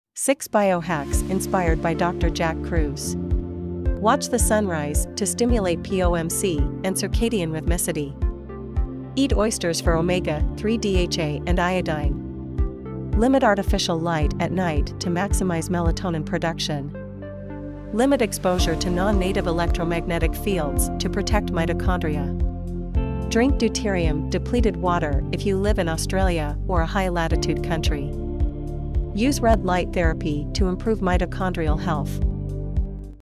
6 Bio Hacks Inspired by Dr Jack Kruse
6 Bio hacks inspired by Dr Jack Kruse

In this video, I'm sharing 6 Bio Hacks inspired by Dr. Jack Kruse, a world-renowned expert on blue light exposure and melatonin.

If you're interested in learning more about how blue light can affect your health, then be sure to check out Dr. Jack Kruse's podcast - Episode 16 of the Gut Health Gurus Podcast. In this series , Dr. Jack Kruse shares his insights on a variety of topics, including blue light and melatonin.

If you're ready to start taking your health and wellness seriously, be sure to check out Dr. Jack Kruse's work! These Bio Hacks are sure to help you live a healthier, more balanced life!